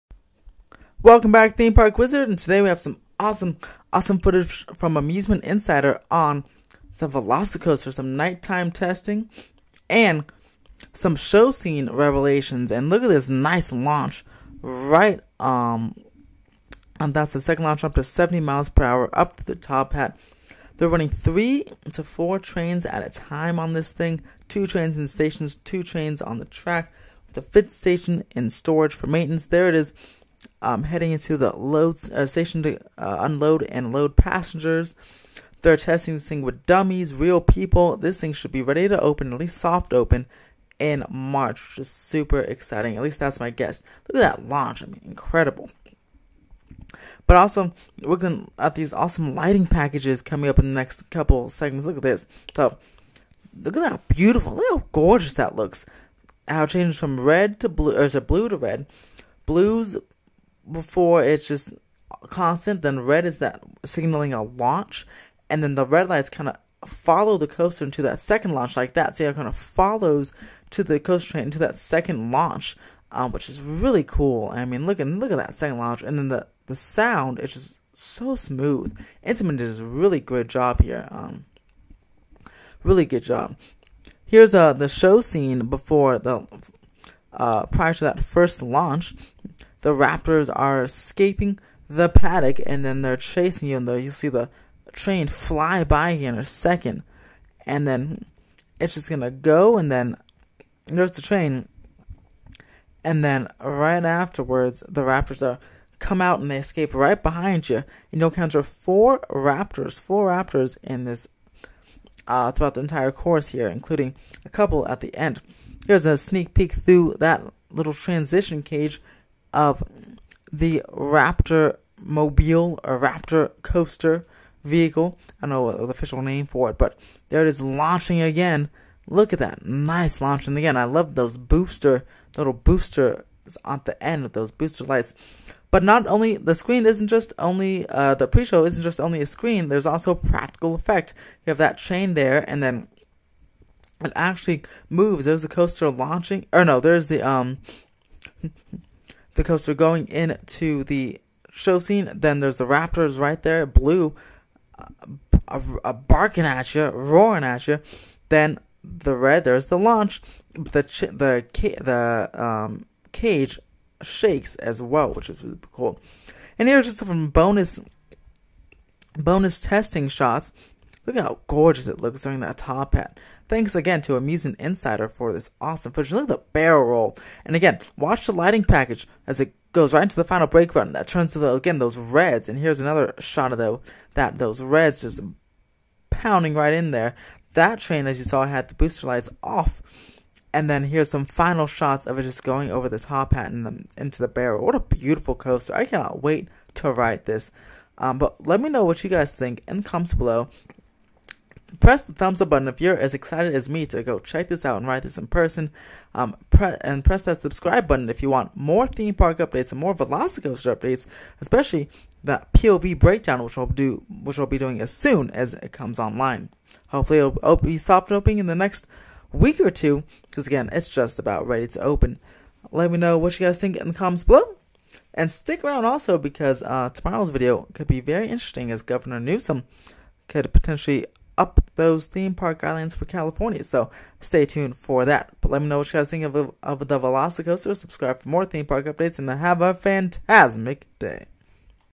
Velocicoaster Show Scene and Show Lighting REVEALED! | Islands of Adventure | Universal Orlando
Thanks to @AmusementInsider we have some amazing night time footage of the velocicoaster and its show scenes and show lighting!